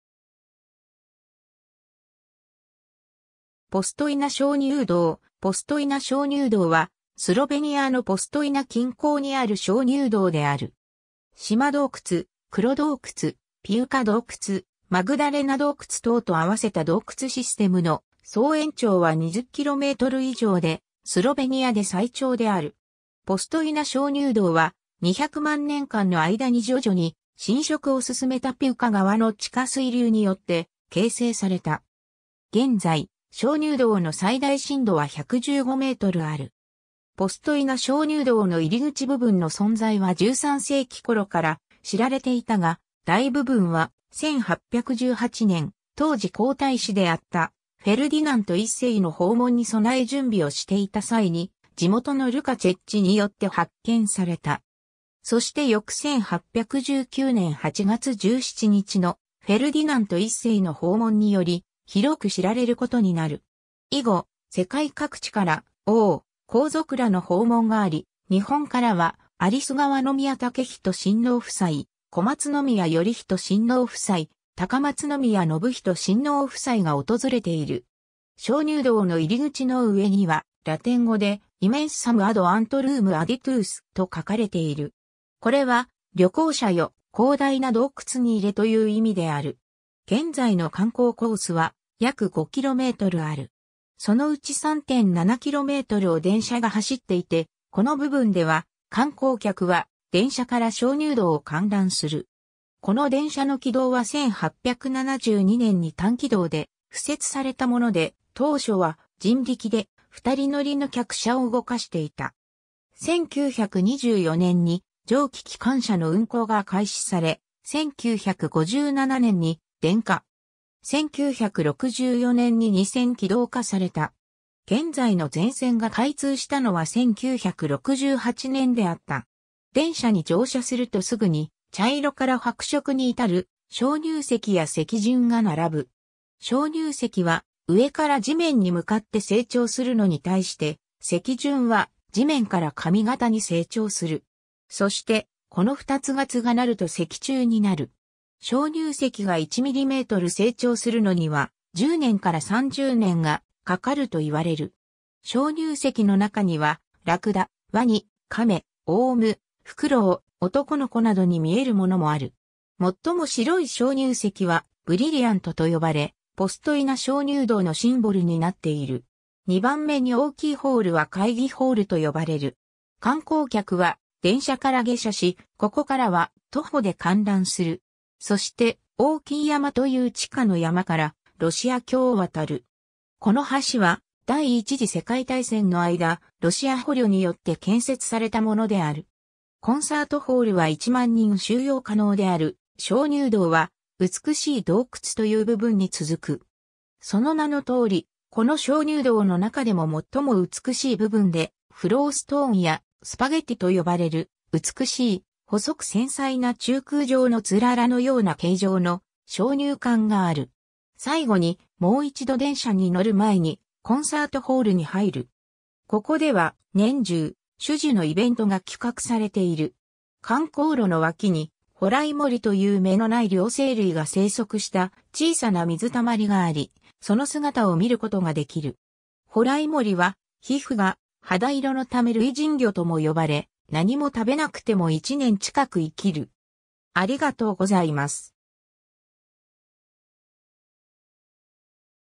ポストイナ鍾乳洞
ポストイナ鍾乳洞 ポストイナ鍾乳洞（ポストイナしょうにゅうどう、Postojnska Jama）は、スロベニアのポストイナ近郊にある鍾乳洞である。
島洞窟、黒洞窟、ピウカ洞窟、マグダレナ洞窟等と合わせた洞窟システムの総延長は20km以上で、スロベニアで最長である。
 ポストイナ鍾乳洞は、200万年間の間に徐々に侵食を進めたピウカ川の地下水流によって形成された。
現在、鍾乳洞の最大深度は115mある。
 ポストイナ鍾乳洞の入口部分の存在は13世紀頃から知られていたが、大部分は、1818年、当時皇太子であったフェルディナント1世の訪問に備え準備をしていた際に、地元のルカ・チェッチによって発見された。
そして翌1819年8月17日のフェルディナント1世の訪問により、広く知られることになる。
以後、世界各地から王/皇族らの訪問があり、日本からは有栖川宮威仁親王夫妻（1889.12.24）、小松宮依仁親王夫妻（1894.4.3）、高松宮宣仁親王夫妻（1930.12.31）が訪れている。
 鍾乳洞の入口の上には、ラテン語で"Immensum ad antrum aditus!"と書かれている。
これは「旅行者よ、広大な洞窟に入れ」という意味である。
 現在の観光コースは約5kmある。
そのうち3.7kmを電車が走っていて、この部分では観光客は電車から鍾乳洞を観覧する。
この電車の軌道は1872年に単軌道で敷設されたもので、当初は人力で2人乗りの客車を動かしていた。
1924年に蒸気機関車の運行が開始され、1957年に電化。
1964年に二線軌道化された。
現在の全線が開通したのは1968年であった。
 電車に乗車するとすぐに、茶色から白色に至る鍾乳石や石筍が並ぶ。
鍾乳石は上から地面に向かって成長するのに対して、石筍は地面から上方に成長する。
そして、この2つがつがなると石柱になる。
鍾乳石が1mm成長するのには、10年から30年がかかるといわれる。
鍾乳石の中には、ラクダ，ワニ、カメ、オウム、フクロウ、男の子などに見えるものもある。
最も白い鍾乳石は、「ブリリアント」（Brilliant）と呼ばれ、ポストイナ鍾乳洞のシンボルになっている。
 二番目に大きいホールは会議ホール（Congress Hall）と呼ばれる。
観光客は電車から下車し、ここからは徒歩で観覧する。
そして、大きい山（Great Mountain）という地下の山からロシア橋（Russian Bridge）を渡る。
この橋は、第一次世界大戦の間、ロシア捕虜によって建設されたものである。
 コンサートホールは１万人収容可能である 鍾乳洞は、美しい洞窟（Beautiful caves）という部分に続く。
その名のとおり、この鍾乳洞の中でも最も美しい部分で、フローストーンや、「スパゲティ」と呼ばれる美しい細く繊細な中空状の氷柱のような形状の鍾乳管がある。
 最後に、もう一度電車に乗る前に、コンサート・ホール（Concert Hall）に入る。
ここでは、年中、種々のイベントが企画されている。
 観光路の脇に、ホライモリという目のない両生類が生息した小さな水溜りがあり、その姿を見ることができる。
ホライモリは皮膚が肌色のため類人魚とも呼ばれ、何も食べなくても1年近く生きる。